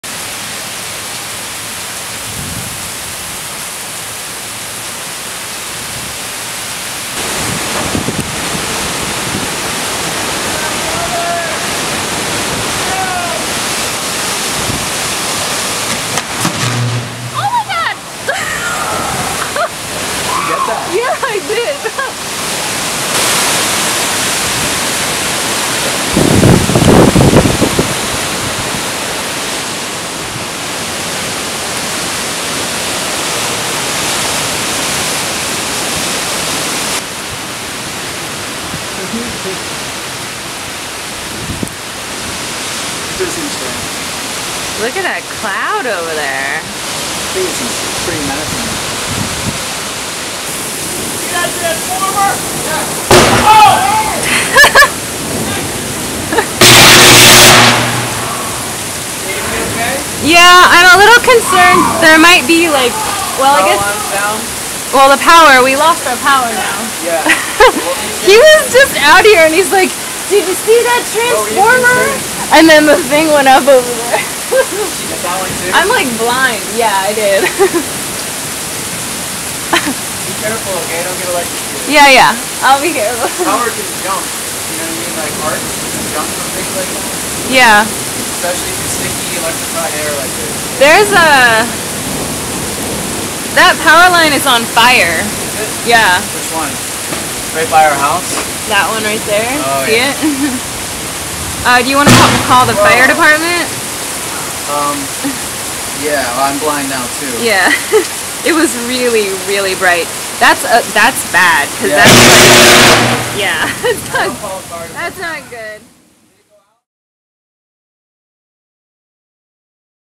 Tornado in Harrisburg
We don't get tornadoes in Harrisburg, PA.  At least we never used to.  Thanks to the effects of climate change, we get them now.  Hello tornadoes, goodbye trees.